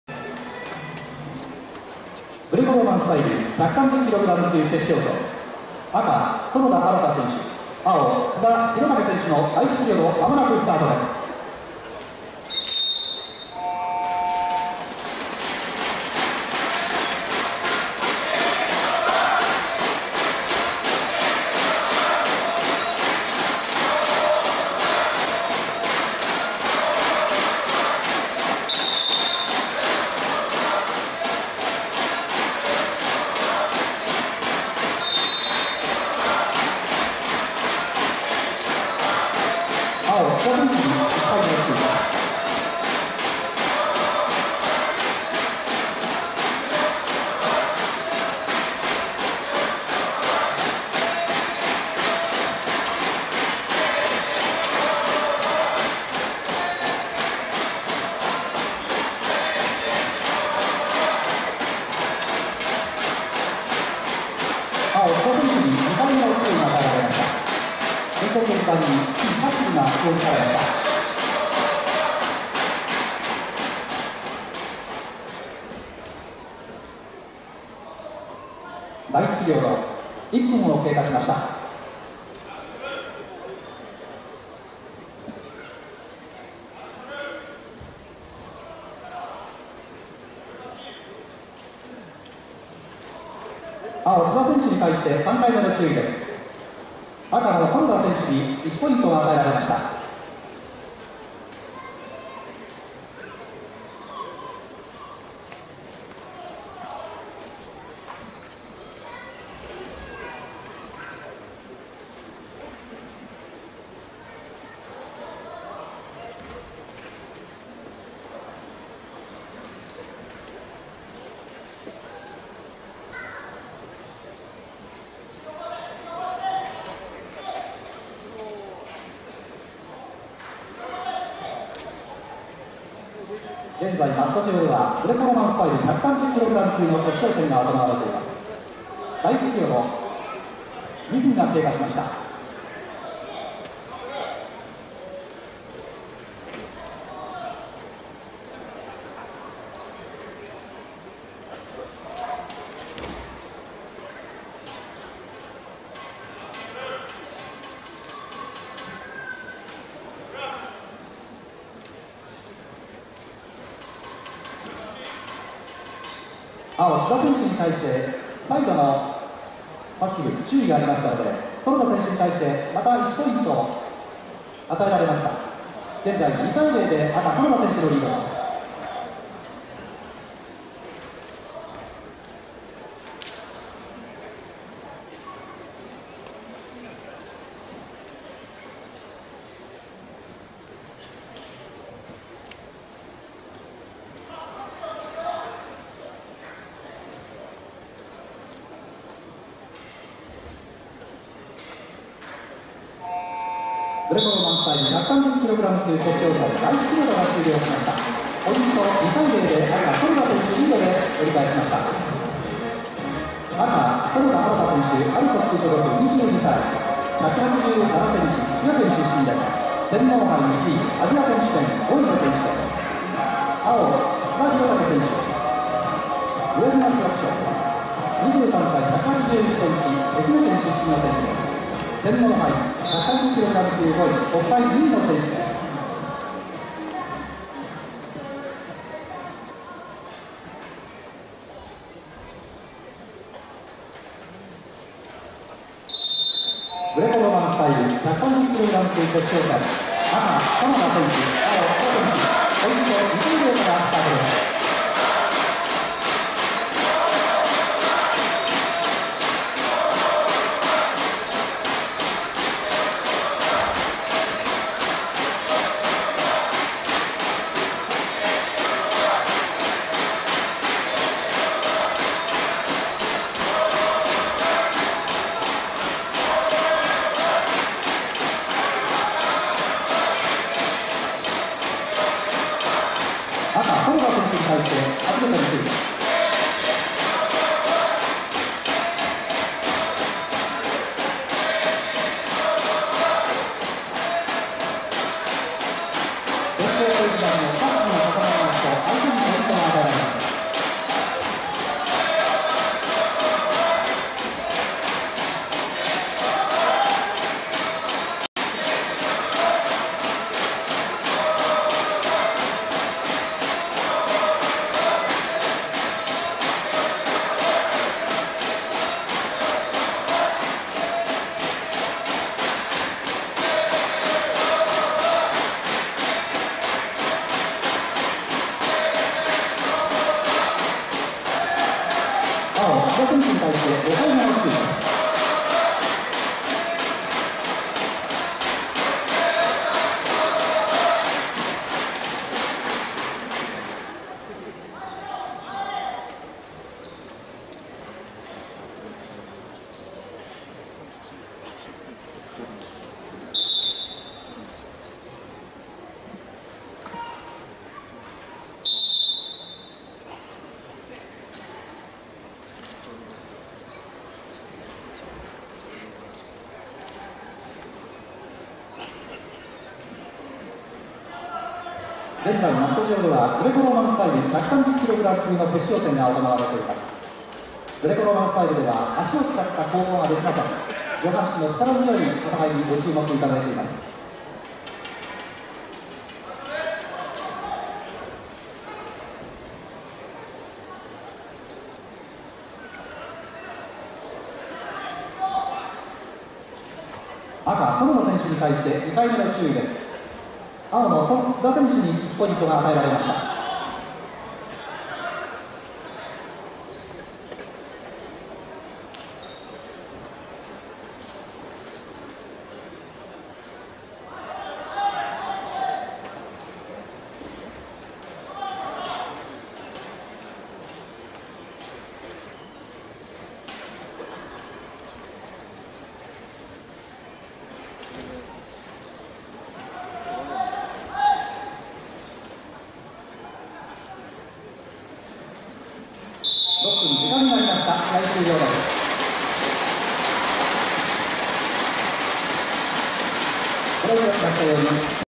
平成29年明治杯全日本選抜選手権【 男子グレコローマンスタイル130kg級決勝】
【130ｋｇ級決勝】園田新（赤＝ＡＬＳＯＫ）○［２－１］●津田大健（宇和島市役所）